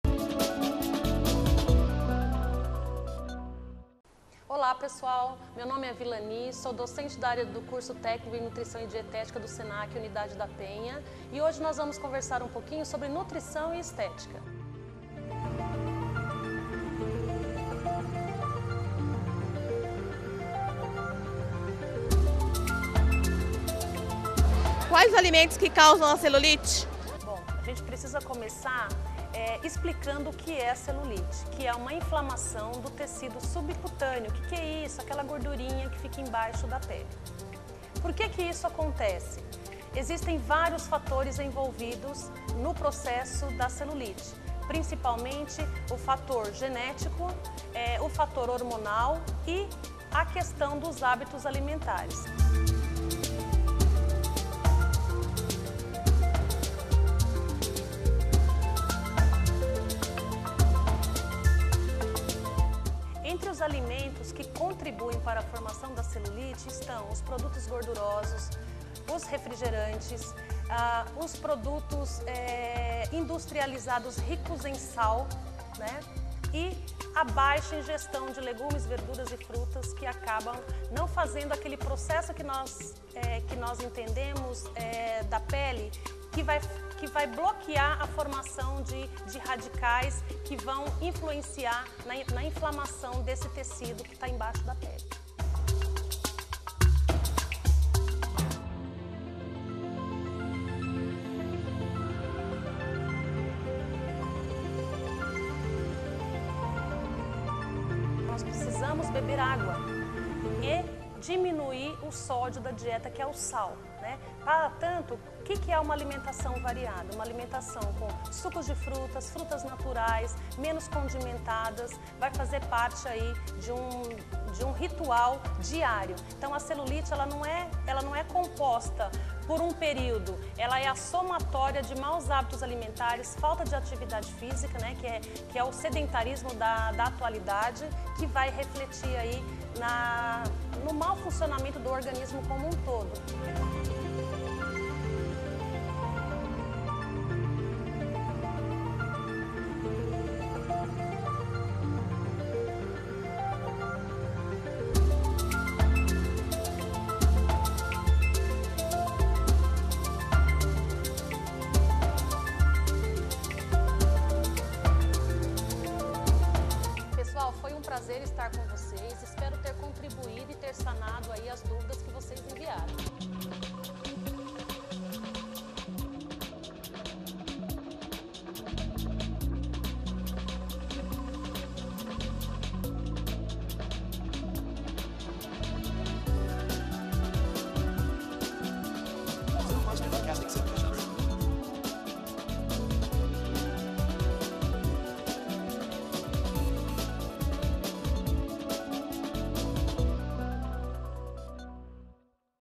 Alimentos que causam a celulite
Os alimentos podem tanto beneficiar a nossa saúde como também prejudicar alguns aspectos e funções do nosso corpo. Um exemplo disso é a ingestão de ingredientes que causam a celulite. A docente em nutrição e dietética Vilani Dias dá dicas para melhorar a alimentação.